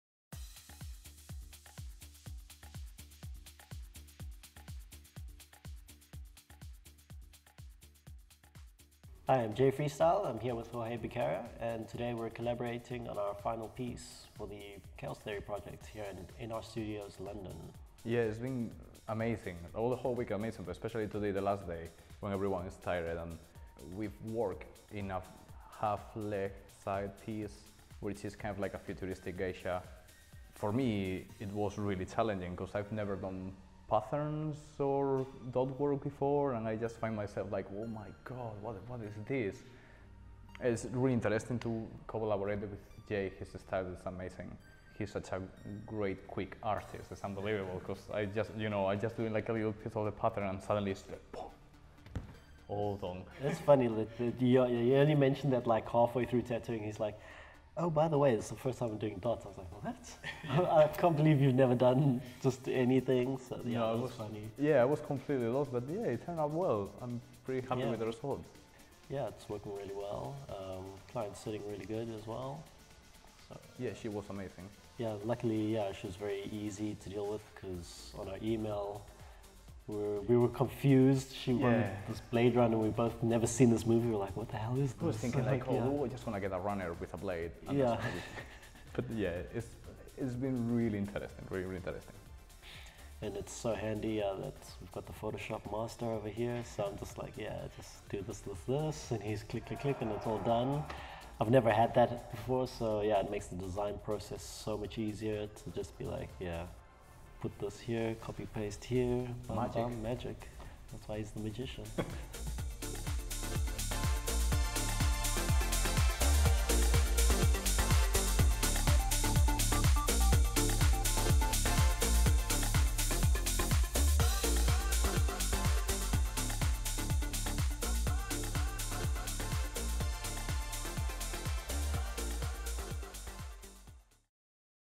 Jorge Becerra & Jay Freestyle Interview - The Kaos Theory Project | Tattoo Artist Collaboration
Jorge Becerra and Jay Freestyle combined their talents to tattoo an awesome Geisha on their client.

Tattoo Artists: Jorge Becerra & Jay Freestyle